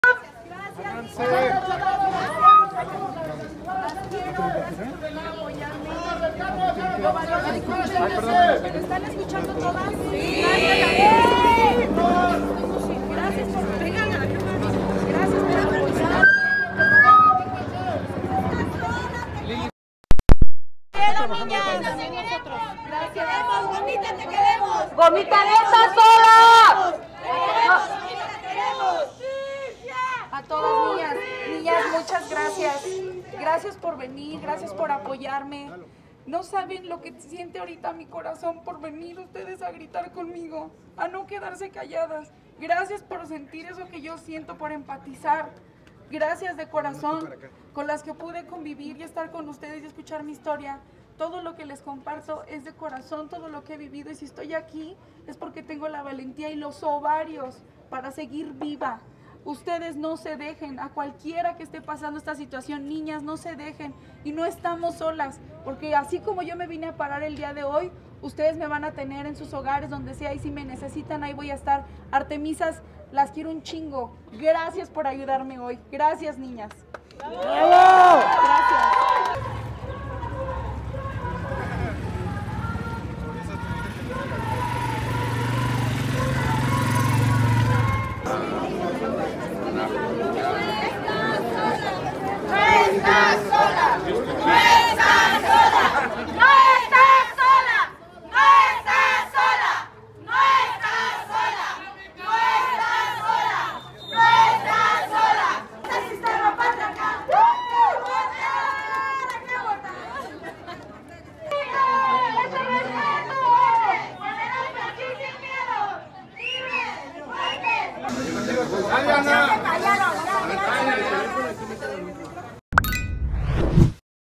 🚨GOMITA LLORA Y LE AGRADECE A FANS QUE LA ACOMPAÑARON A DENUNC1AR A SU PADRE🚨
Gomita acudió el día de ayer a la fiscalía a denunciar a su padre y un grupo de mujeres estuvo con ella en el lugar para extenarle su apoyo en estos difíciles momentos.

SÍGUEME